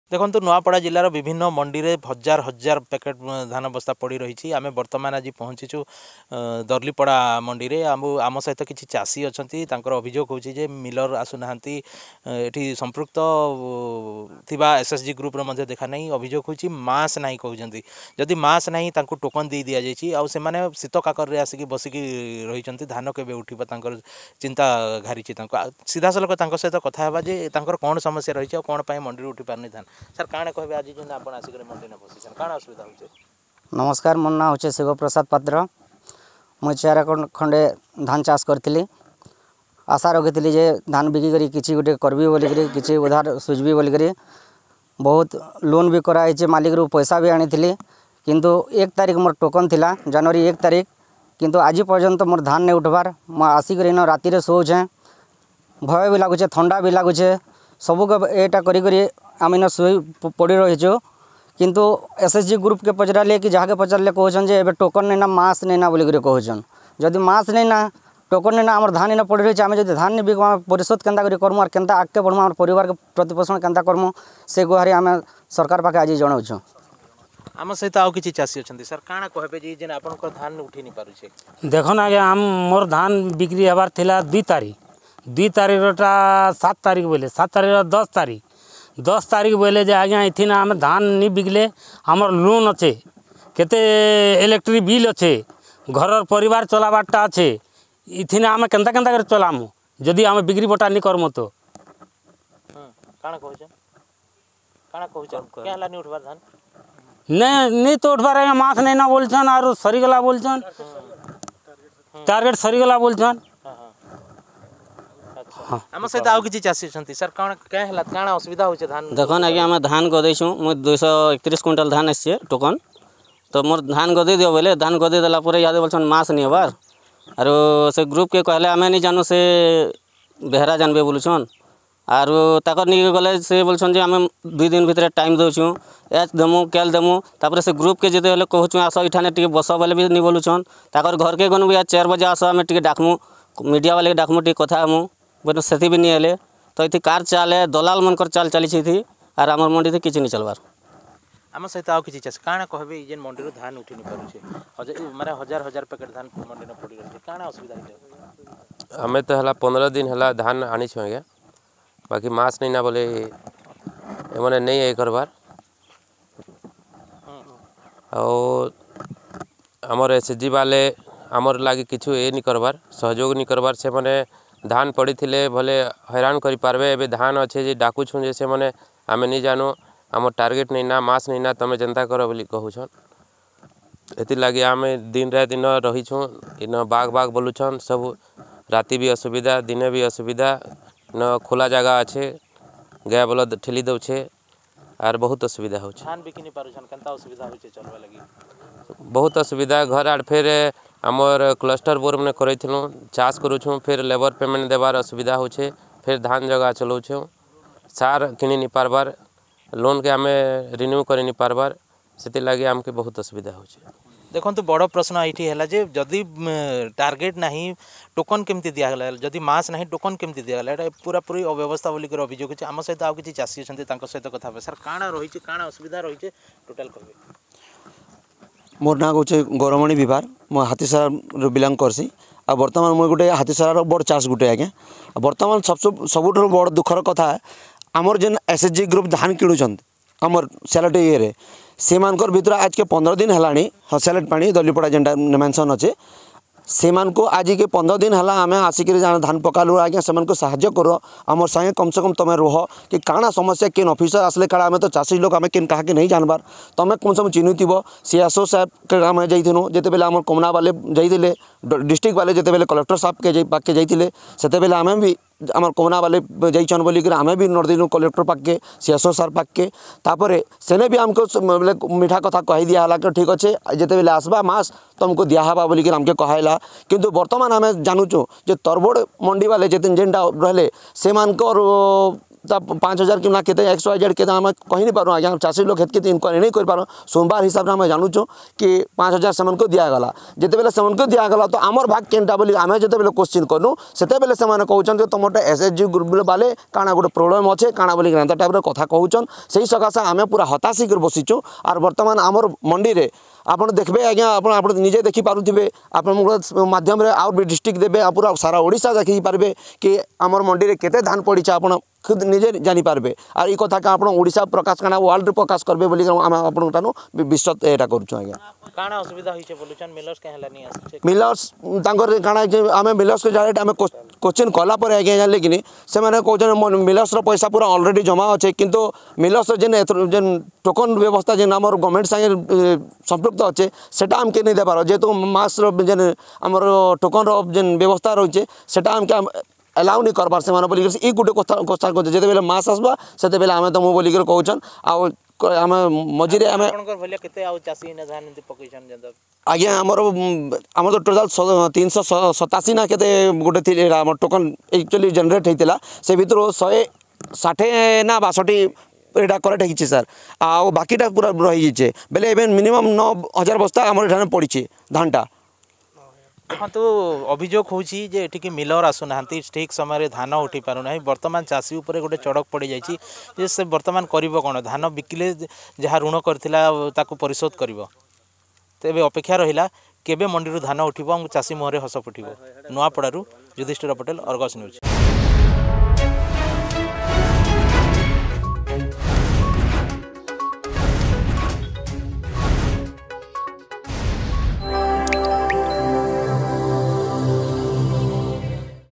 ନୂଆପଡା ସ୍ଵୟଂ ସହାୟକ ଗୋଷ୍ଠୀର ମନମାନି
ନୂଆପଡା ସ୍ଵୟଂ ସହାୟକ ଗୋଷ୍ଠୀର ମନମାନି ଯୋଗୁଁ ସିଲେଟ ପାଣି ମଣ୍ଡିରେ ପ୍ରାୟ 10 ହଜାର ବସ୍ତା ଧାନ ପଡ଼ି ରହିଥିବା ବେଳେ ଦୀର୍ଘ ଦଶ ଦିନ ହେବା ଶୀତ କାକରରେ ପଡ଼ି ରହିଛି ଚାଷୀଙ୍କ କଷ୍ଟ ଉପାର୍ଜିତ ଧାନ ...
ଅଧିକ ଖବର ପାଇଁ ଡ଼ାଉନଲୋଡ କରନ୍ତୁ “Argus News” ଆପ୍